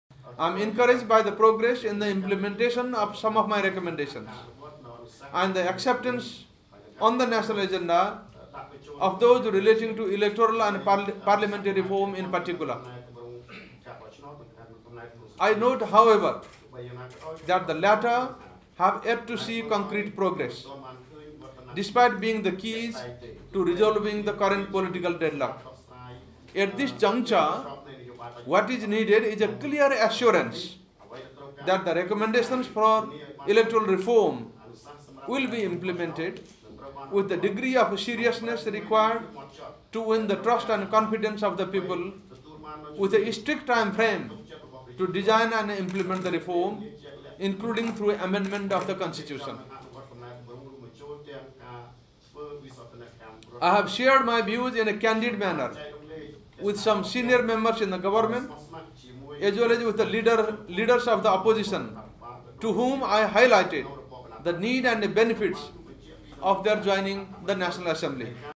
Subedi wraps up latest visit
United Nations Special Rapporteur on the Situation of Human Rights in Cambodia Surya Subedi reinforces the steps he believes are necessary to end Cambodia's political deadlock